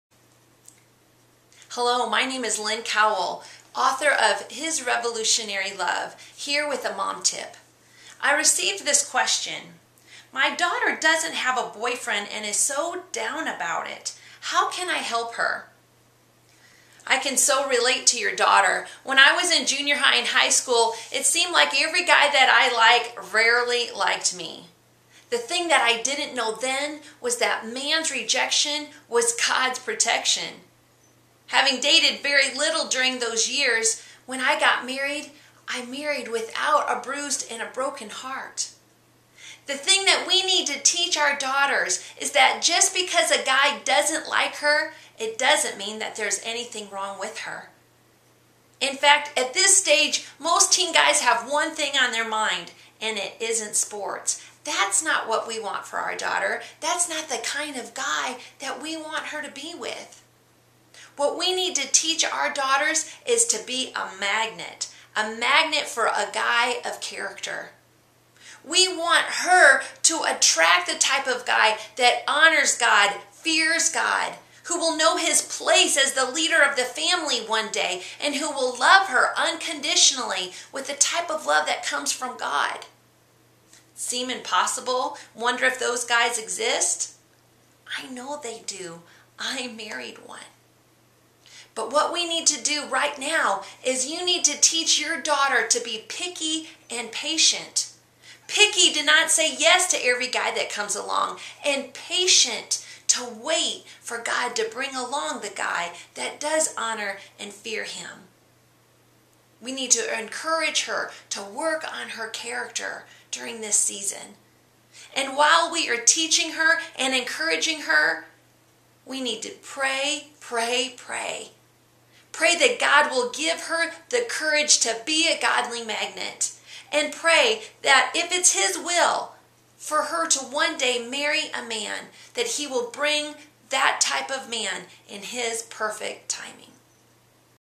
Q & A - My daughter doesn't have a boyfriend and is so down about it. How can I help her?
Lynn Cowell, author of "His Revolutionary Love" answers the question for moms, "My daughter doesn't have a boyfriend and is down about it. How can I help her?